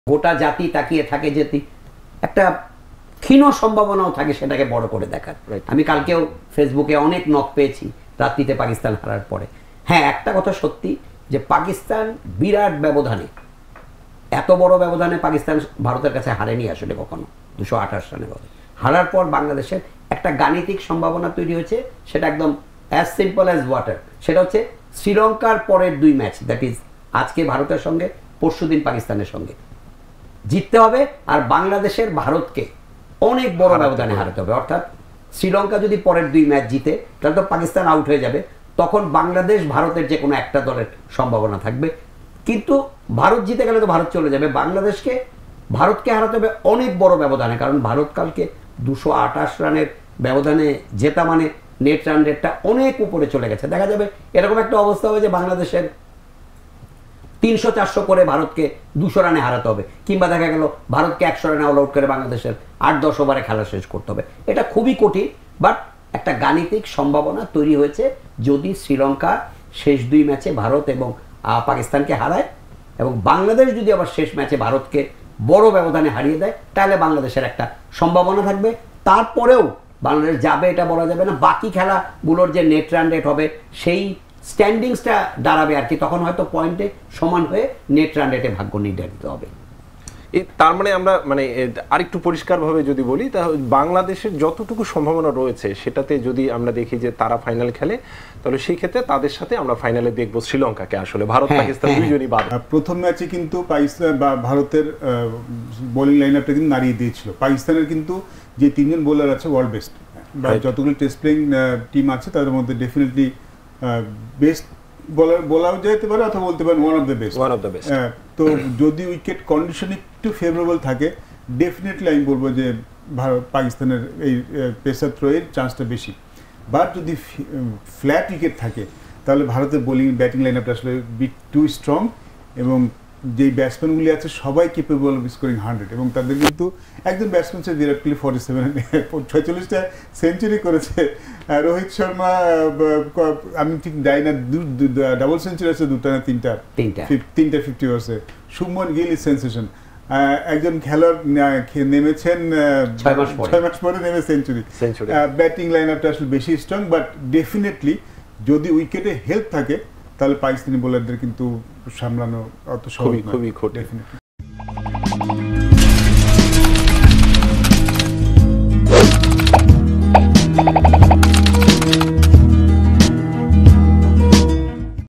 কোন সমীকরণে এখনও বাংলাদেশের ফাইনালে ওঠার সম্ভাবনা রয়েছে? || Asia Cup 2023 || Jago News
কোন সমীকরণে এখনও বাংলাদেশের ফাইনালে ওঠার সম্ভাবনা রয়েছে? || Asia Cup 2023 |